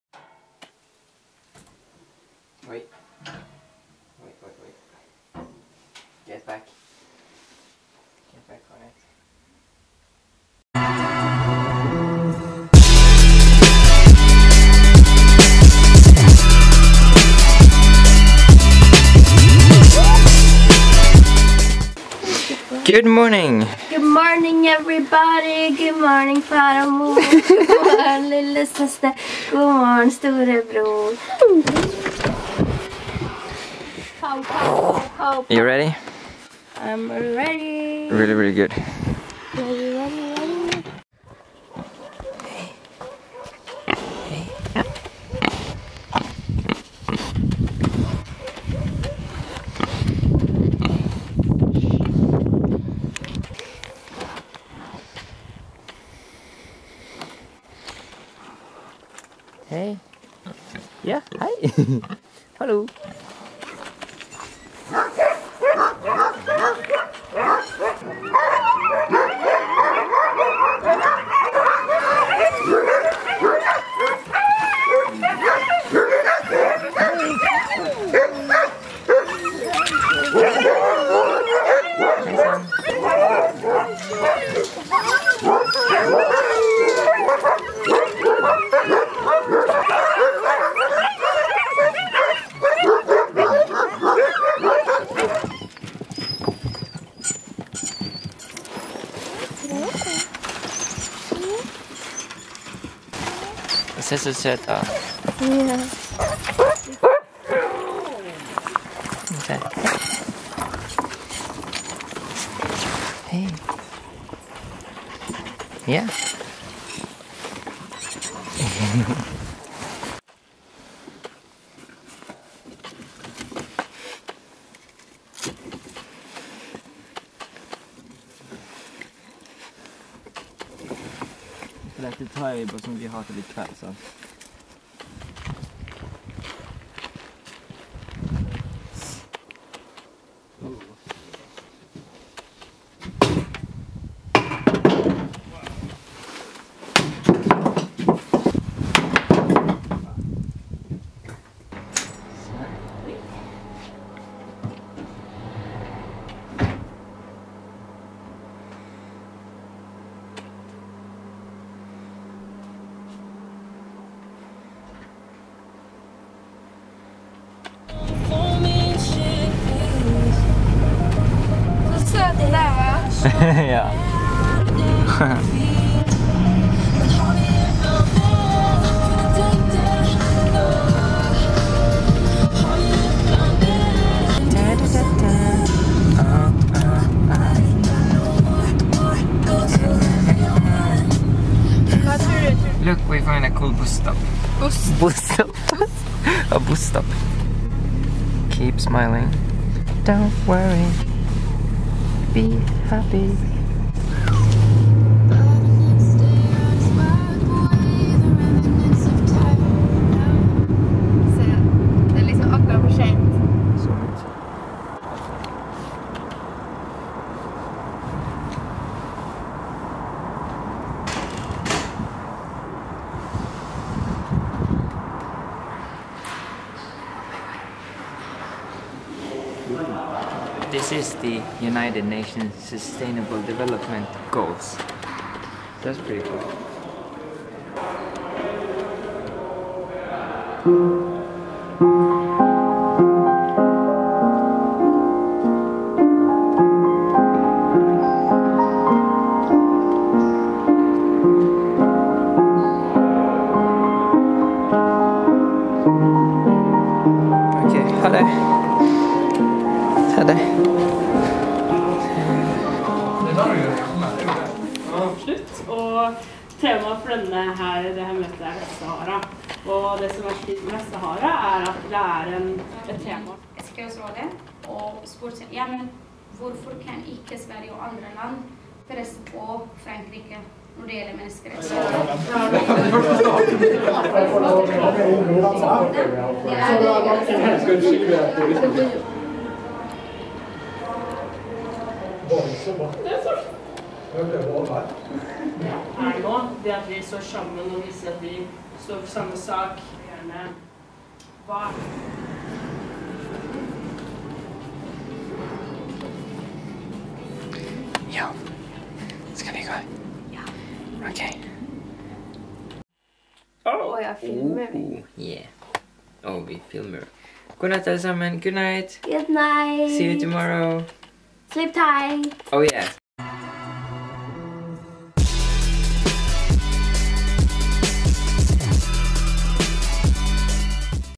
Short vlog from Ålesund | SkaDaily 050
Here is a short vlog from Ålesund. We were lucky enough to stay with our friend, Andrea's aunt and uncle, in their little guest house. Cuddle with pigs and dogs❤︎

Anything else? Hit me up!

Today's editor: Andrea aka. @mostavokado on Instagram! Thank you Andrea!❤︎